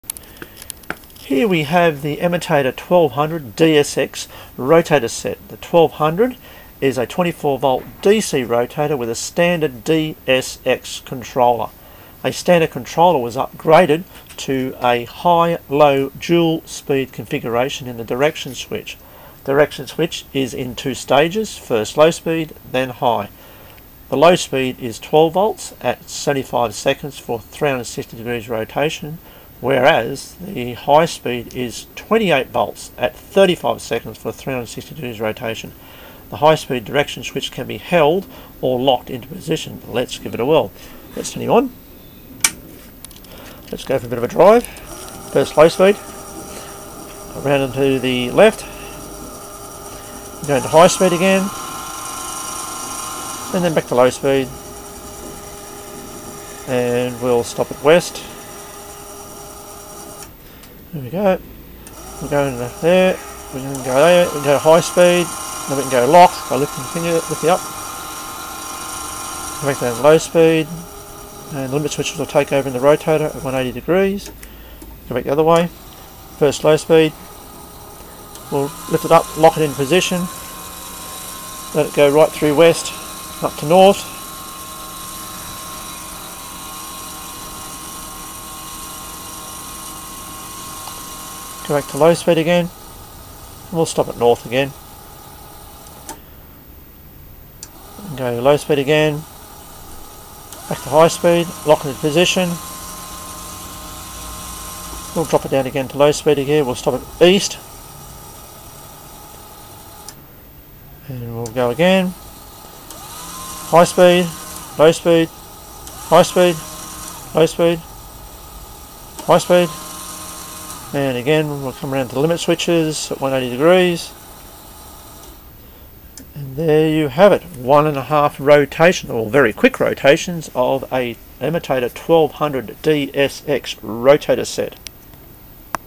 Emotator 1200 DS Dual Speed
Here we have the Emotator 1200 DSX rotator set.
The 1200 is a 24 volt DC rotator with a standard DS controller.
A standard controller was upgraded to a high/low  DUAL SPEED configuration in the direction switch.
The direction switch is in two stages, first low speed then high.
Low speed is 12 volts @ about 75 seconds for 360 degrees rotation, where as the high speed is 28 volts @ about 35 seconds for 360 degrees rotation.
The high speed direction switch can be held for momentary operation or locked onto position for hands free operation.
The controller also has been upgraded with LED display illumination with a new compass display. Plus the back panel has been upgraded as well with a fused IEC socket, 8 pin Cinch socket & earth bolt with wing nut.
The 1200 rotator has been full refurbished plus been upgraded with a 5 pin IP67 socket & plug.

For rotator sale, repairs & spares etc, please visit my web or contact Peter of Duoro Services via the contact page.